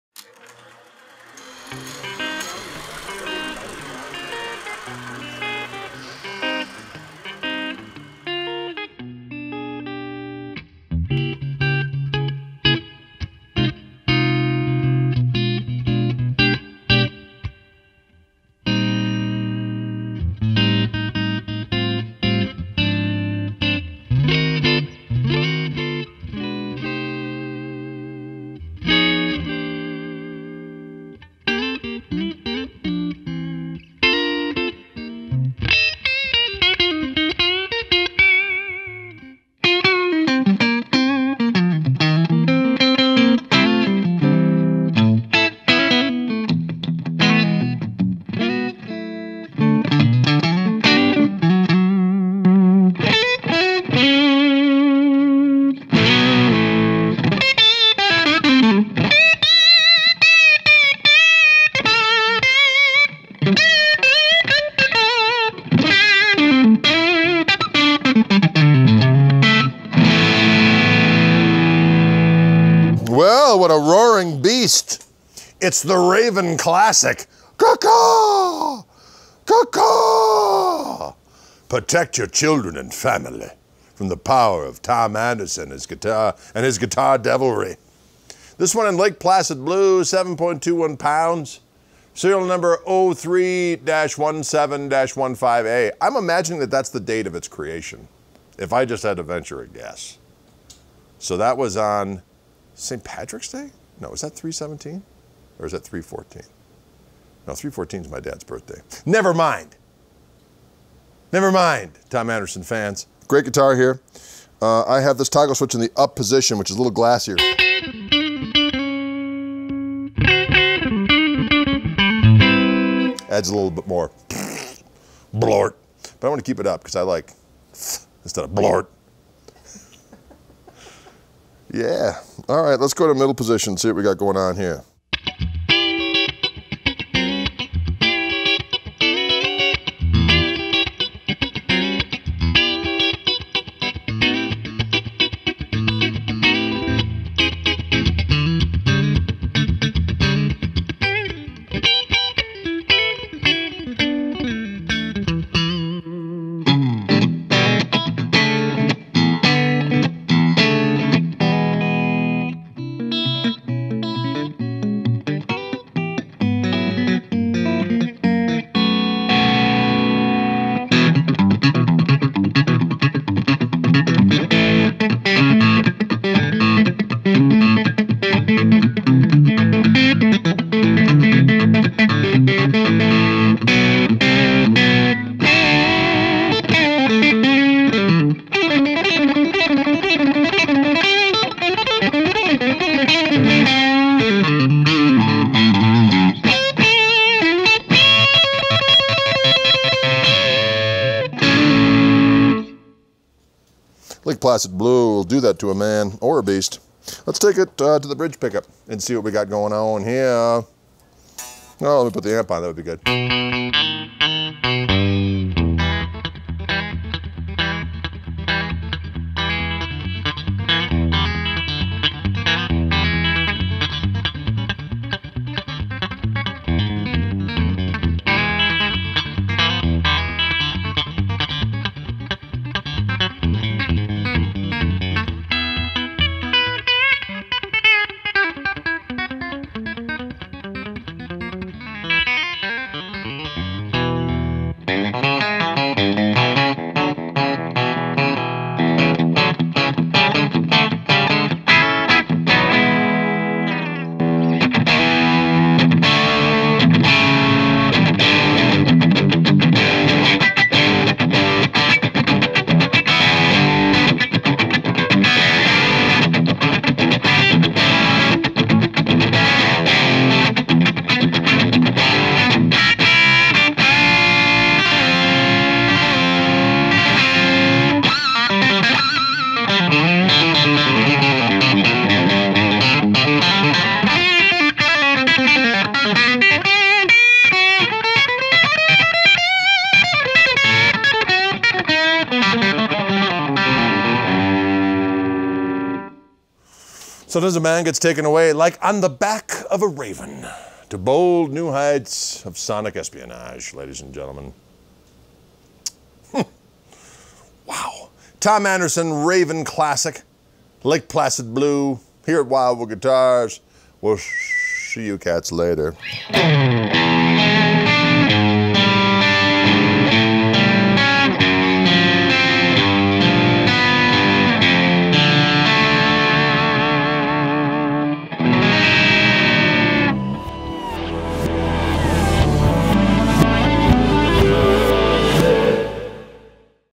Tom Anderson Raven Classic • SN: 03-17-15A
A stunning guitar with ease of play and that classic chime and sparkle!  Check out this Tom Anderson Raven Classic played by the irreverent Greg Koch!

Serial Number: 03-17-15A

Brand: Tom Anderson
Model: Raven Classic
Finish Color: Lake Placid Blue
Weight: 7.21 lbs.
Body Wood: Alder
Neck Wood: Maple
Neck Shape: Even Taper +.030
Neck Dimensions: .840 1st - .900 12th
Fingerboard: Rosewood
Nut Material: 1 11/16"
Frets: Heavy
Pickups: PQ1, PQ2
Controls: 1 Volume, 1 Tone, 3-way Toggleswitch, Cut Switching
Hardware : Chrome
Tailpiece: Vintage Tremolo
Tuners: Locking
Case: Black Hardshell Case